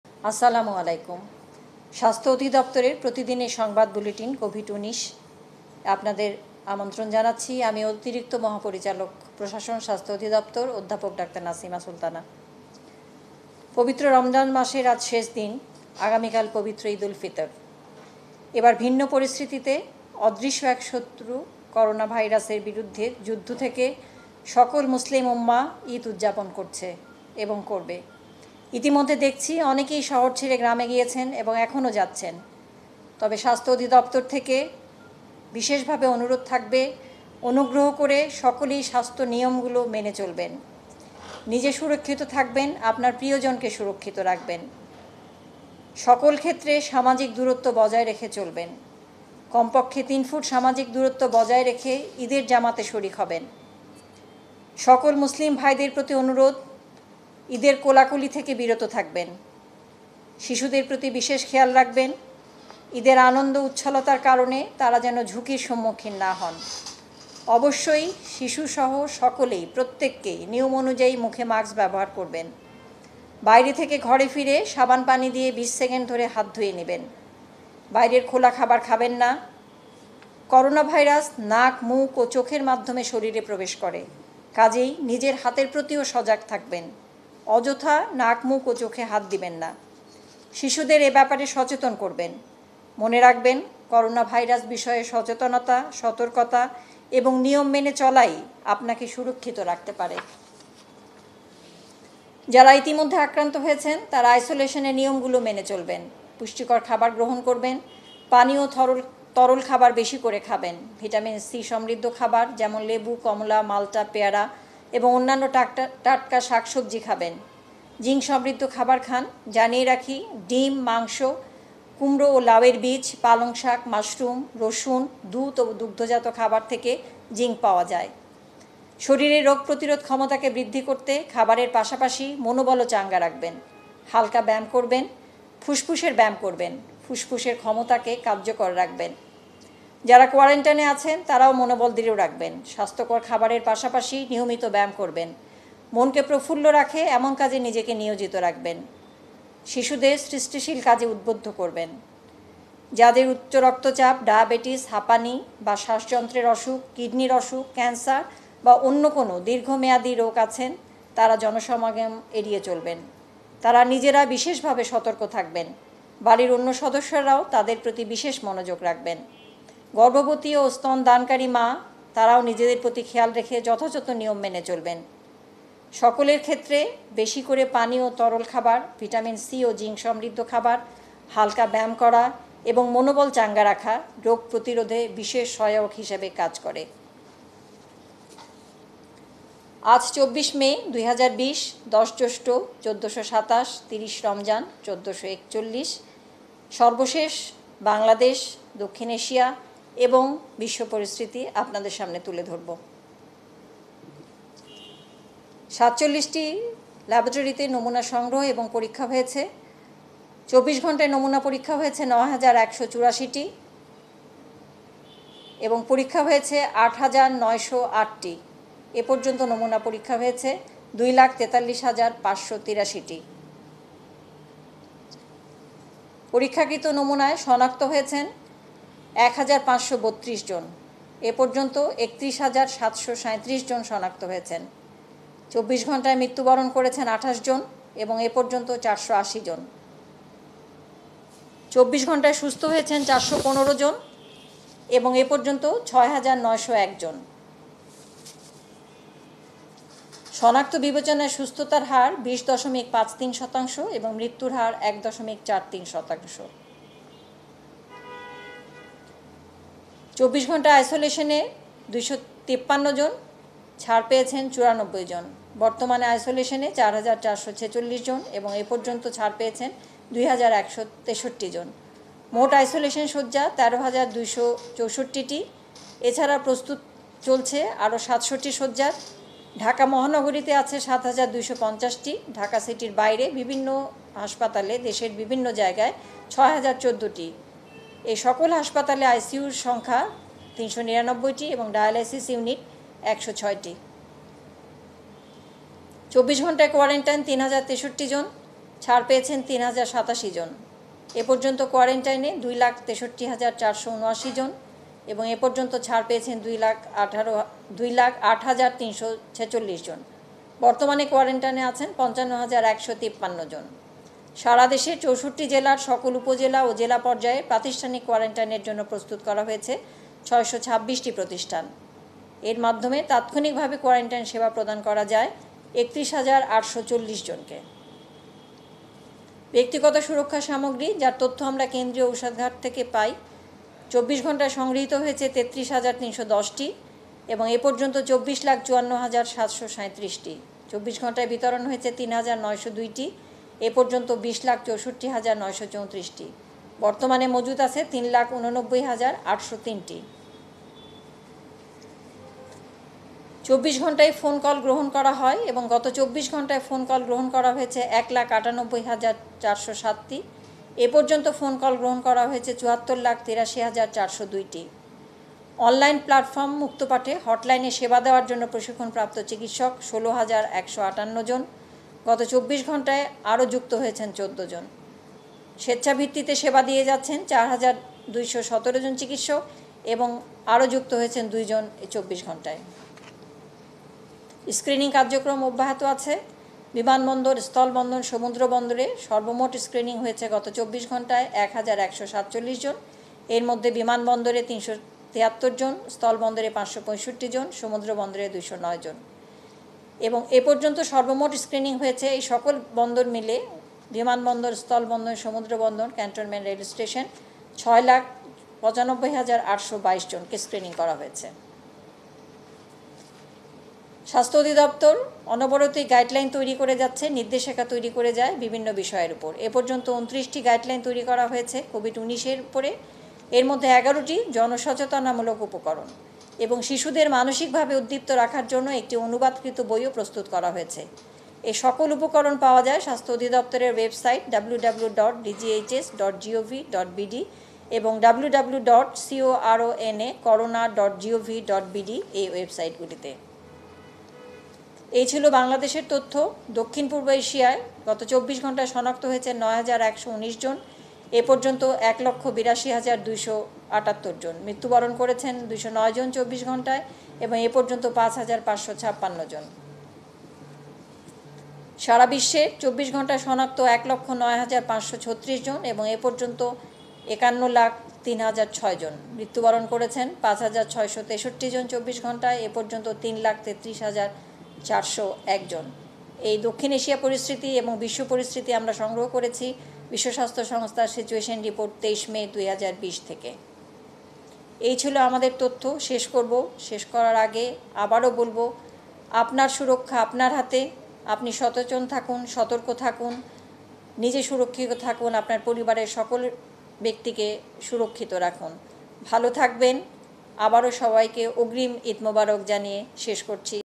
করোনায় ২৪ ঘণ্টায় ২৮ মৃত্যু, শনাক্ত ১৫৩২ | করোনাভাইরাস (COVID-19) UPDATE | 24 May। ATN Bangla News
করোনায় ২৪ ঘণ্টায় ২৮ মৃত্যু, শনাক্ত ১৫৩২ | IEDCR PRESS BRIEFING | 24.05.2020 | ATN Bangla News